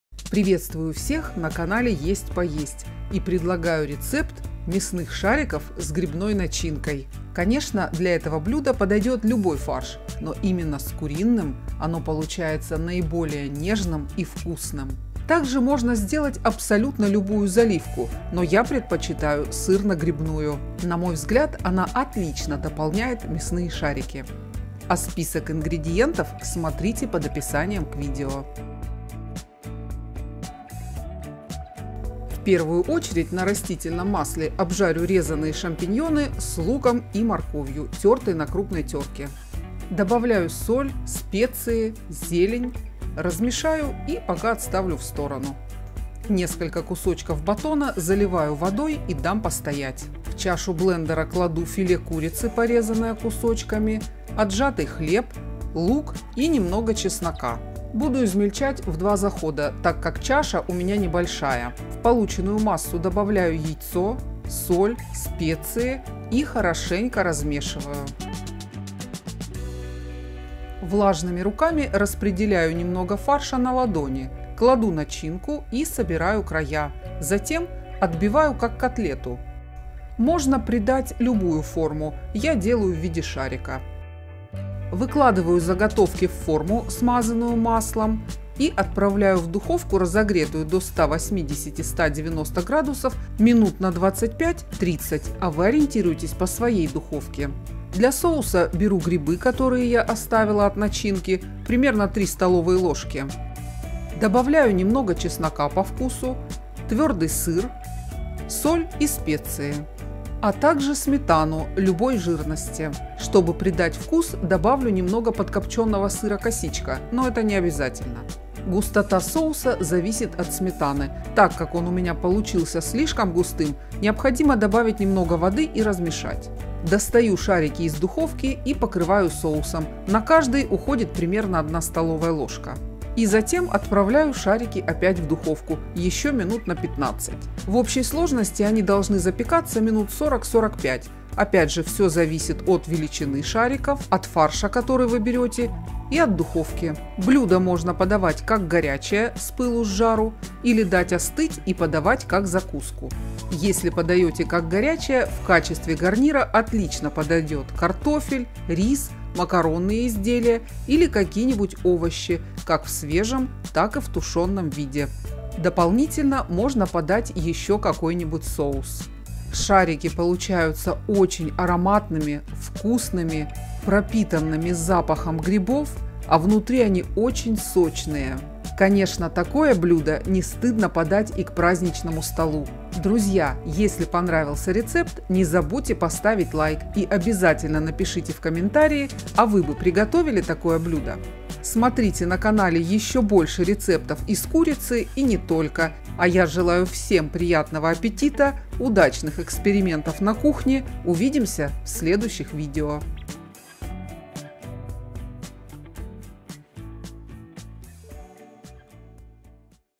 🐔Куриные шарики с грибной начинкой под сырно-грибным соусом.
Великолепные шарики из куриного фарша с начинкой из грибов, запечённые под сырно-грибным соусом. Нежные, сочные, ароматные – такое блюдо можно приготовить к обычному ужину, но, несомненно, оно достойно и праздничного стола. Можно подавать как в качестве горячего, так и в качестве холодной закуски в остывшем виде.
ИНГРЕДИЕНТЫ
филе куриной грудки 800-850 г,
лук 1 шт,
батон 50 г + вода,
Начинка:
Соус:
грибы обжаренные 3-4 ст.л,
вода 3 ст.л (или другое количество до получения нужной консистенции),